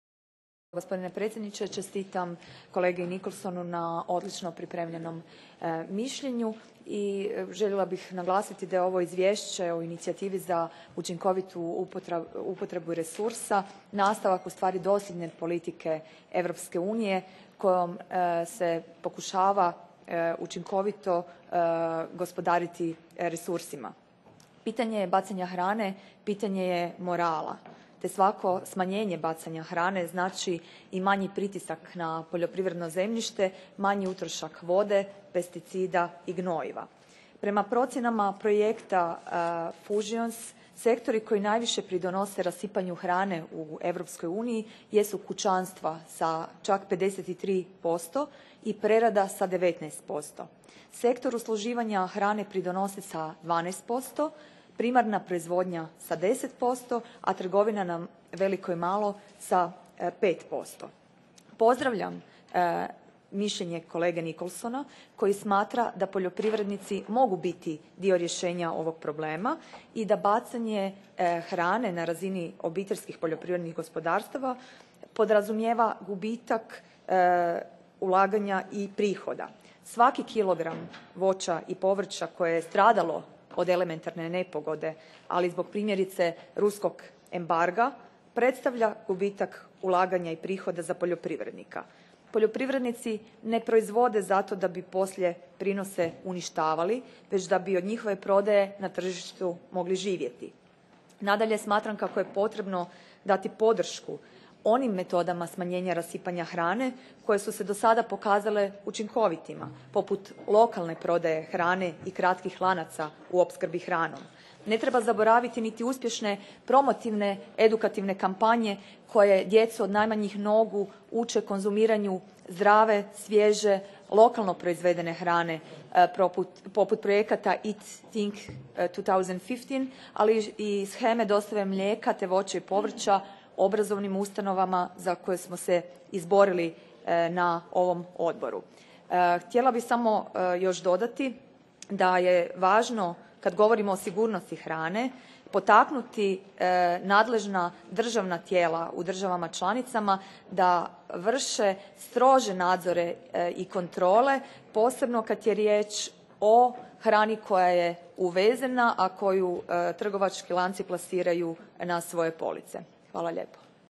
Zastupnici za smanjenje rasipanja hrane
Hrvatska zastupnica u Europskom parlamentu Marijana Petir osvrnula se na Inicijativu o učinkovitoj uporabi resursa – smanjenju rasipanja hrane, poboljšanju sigurnosti hrane kao izvjestiteljica u sjeni u ime EPP-a na raspravi Odbora za poljoprivredu i ruralni razvoj Europskog parlamenta u Bruxellesu rekavši kako je mišljenje izvjestitelja Jamesa Nicholsona nastavak dosljedne politike EU kojom se pokušava učinkovito gospodariti resursima.